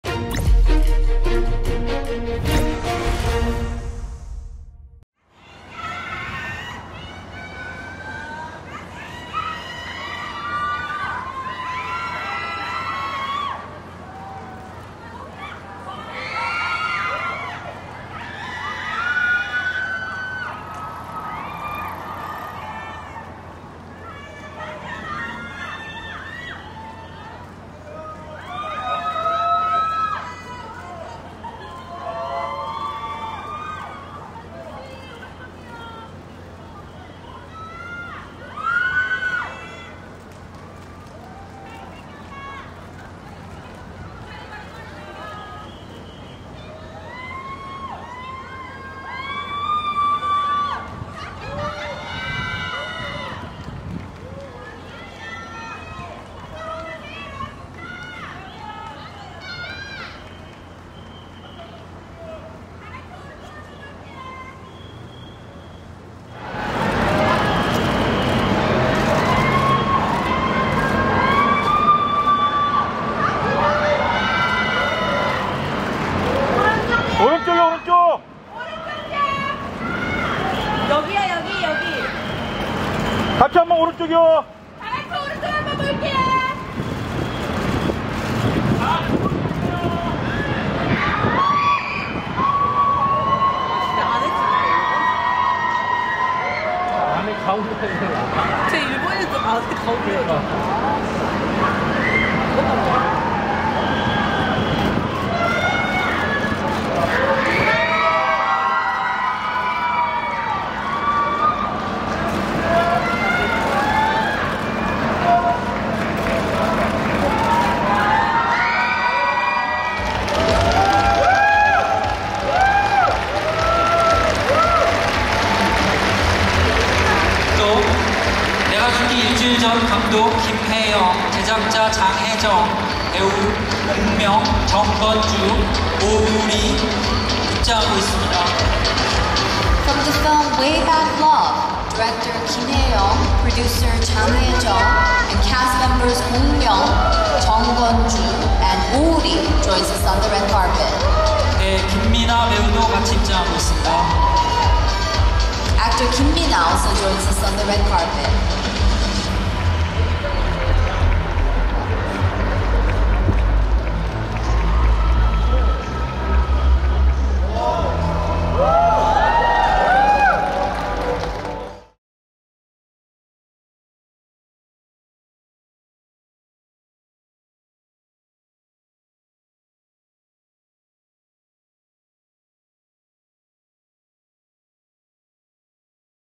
'내가 죽기 일주일 전' (부산국제영화제 레드카펫)
[뉴스인스타 양철수기자] 10월 2일(수) 오후, 부산시 해운대구 우동 영화의 전당에서 제29회 부산국제영화제(BIFF) 레드카펫 행사가 열려 영화 '내가 죽기 일주일 전' 배우 공명, 김민하, 정건주, 오우리가 포토타임을 갖고 있다.

힌퍈 제29회 부산국제영화제는 오는 11일 폐막식까지 영화의 전당을 비롯해 영화제의 발상지인 중구 남포동, 그리고 부산 8곳에서 열흘동안 영화의 향연이 이어진다.

부산국제영화제, 내가 죽기 일주일 전, 공명, 김민하, 정건주, 오우리, 부국제, BIFF, 레드카펫